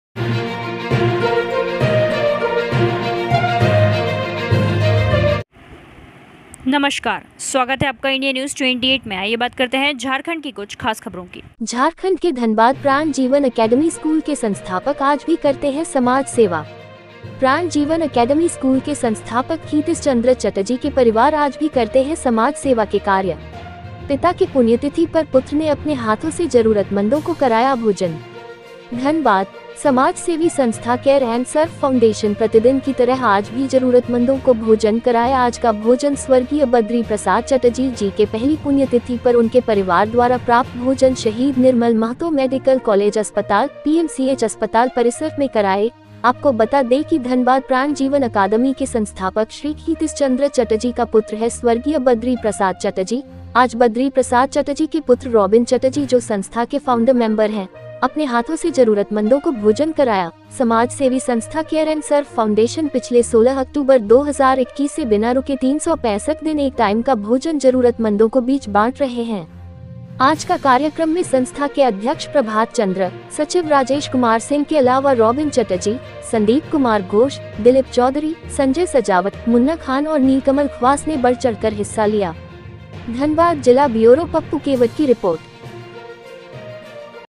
झारखंड के धनबाद प्राण जीवन एकेडमी स्कूल के संस्थापक आज भी करते हैं समाज सेवा
Official